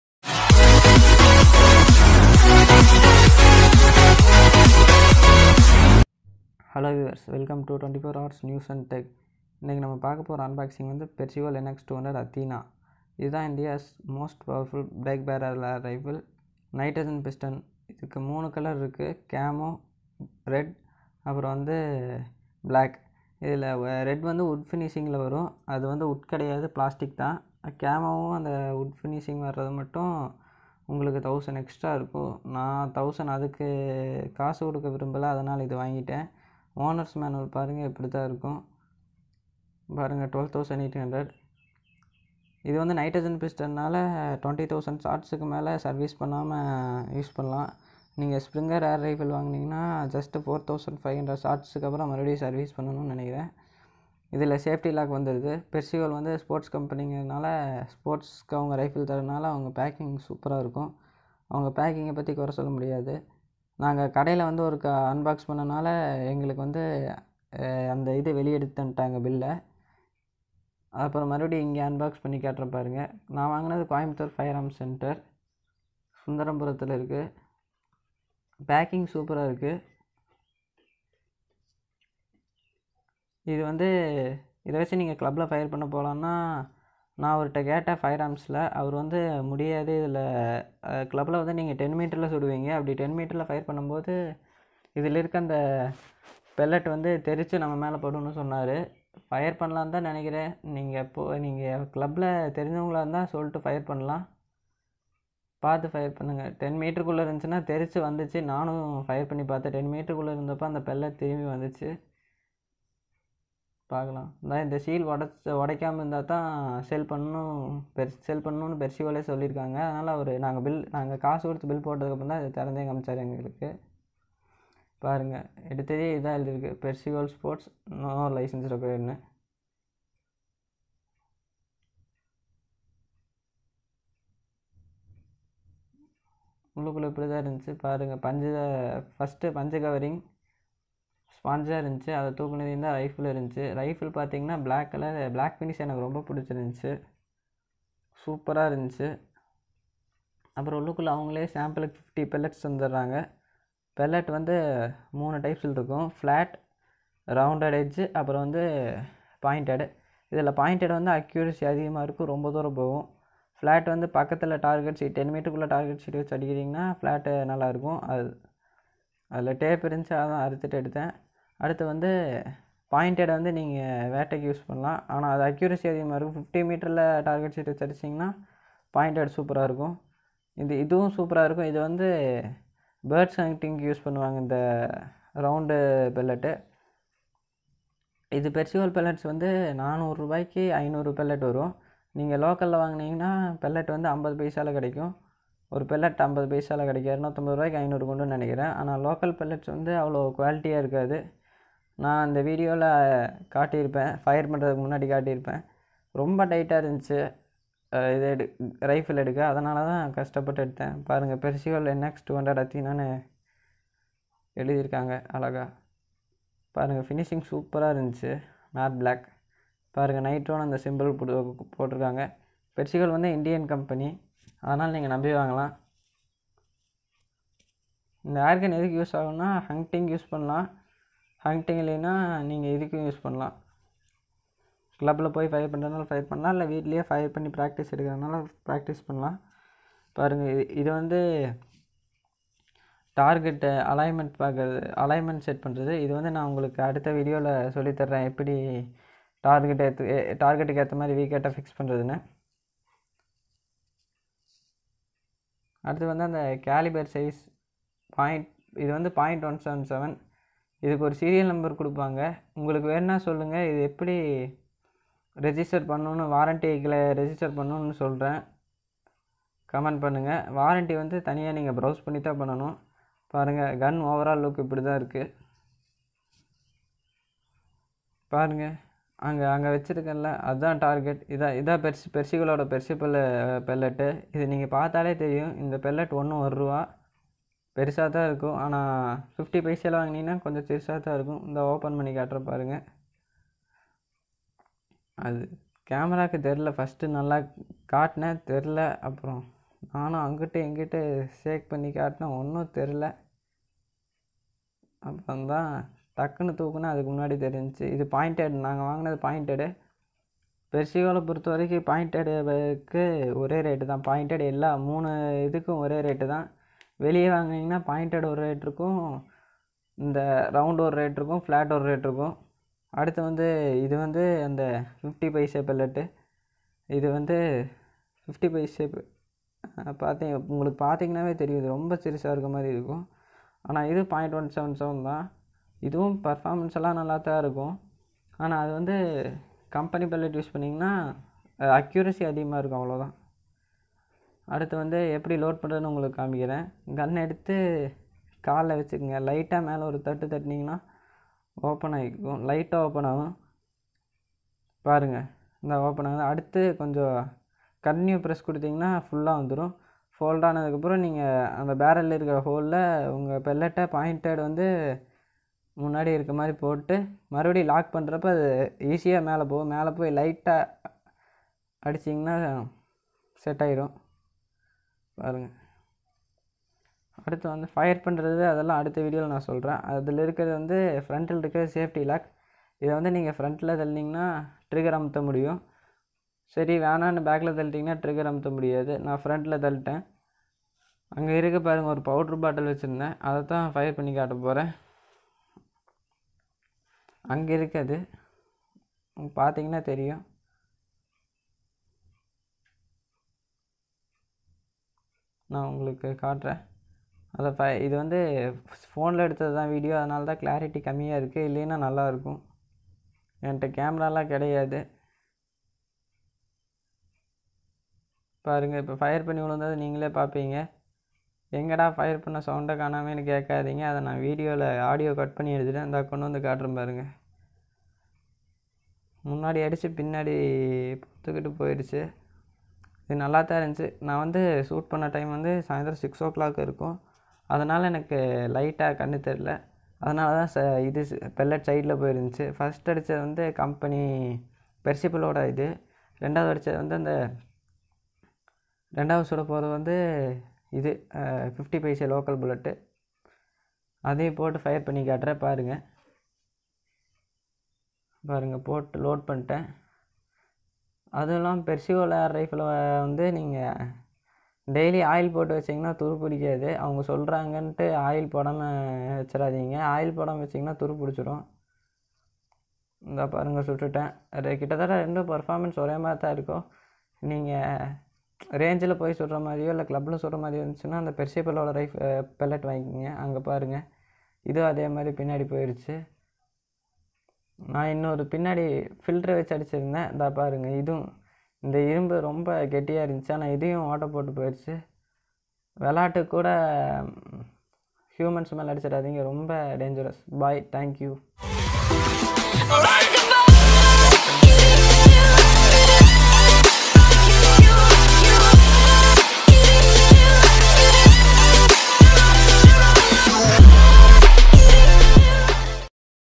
persihole nx 200 Athena air rifle | Unboxing and rewiew in Tamil | India's most powerful air rifle
persihole sports Pvt Ltd
nx 200 Athena
unboxing and rewiew in Tamil
#24 Hours News & Tech

specifications

0.177 Precihole Athena Classic Nitro Piston Break Barrel Air Rifle (NX200 Long Barrel – 860 fps)

The next generation air rifle in the NX series , NX200 Athena is everything you wished for. It's super accurate, classy to look at and packs maximum power within legal limits. It sports a classic style stock with fleur-de-lis checkering, butt cap, butt pad and a metal trigger guard. The air rifle gives you more power with just 30mm increase in length from the NX100 model. It's the perfect companion for fun shooting and open field target shooting - Precise, Powerful and Pretty too.

Specifications:

Description

Athena

Caliber

0.177" (4.5mm)

Velocity

262 mps / 860 fps

Power

20 Joules / 15 ft-lb

Power Plant

Nitro Piston

Action Mechanism

Break Barrel

Barrel Length

450mm (Std) / 370mm (Karbin)

Total Length

1095mm (Std) / 1015mm (Karbin)

Total Weight

3.15 kgs

Barrel Material

Steel

Barrel

Rifled

Front Sight

Post

Rear Sight

Adjustable for windage and elevation

Scopable

Yes

Safety

Automatic

Suggested For

Plinking/Fun/Open Field Target

Mounting Rail

3/8" (11mm) Dovetail

Trigger

Two Stage

Stock Material

Synthetic Black Finish, Synthetic Wood Finish, Synthetic Camo Finish

price 13,000
and 14,000 price depends upon colour